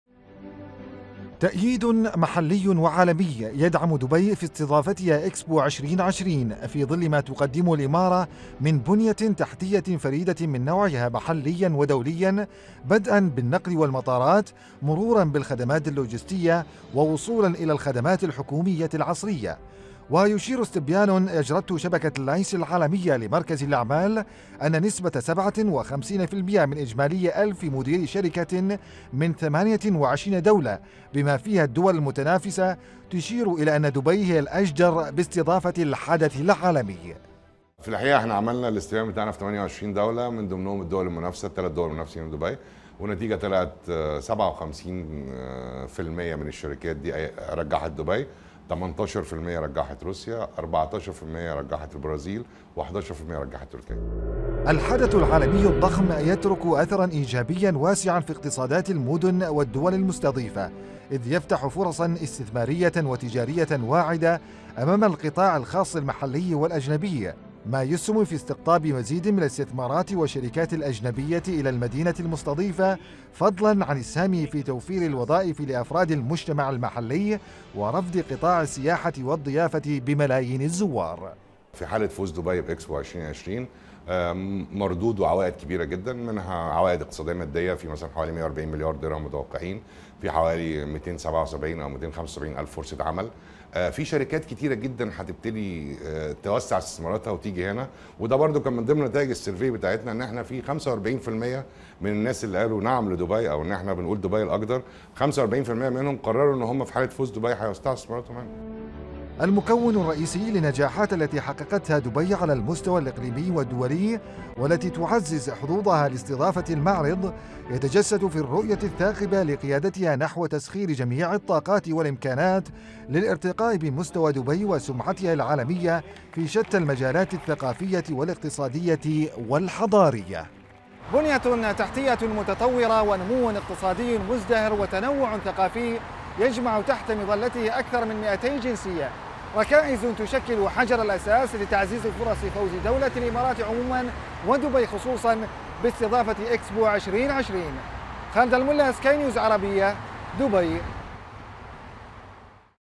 دبي الأوفر حظا باستضافة إكسبو 2020
أظهر استبيان أجرته شبكة اللاينس العالمية لمراكز الأعمال أن نسبة كبيرة من مجتمع الأعمال الدولي تعتبر دبي الأجدر باستضافة معرض اكسبو 2020 مقارنة بالمدن المتنافسة الأخرى. لمشاهدة المزيد، يرجى زيارة صفحة الفيديو على الموقع الإلكتروني لسكاي نيوز عربية من خلال الضغط على الرابط التالي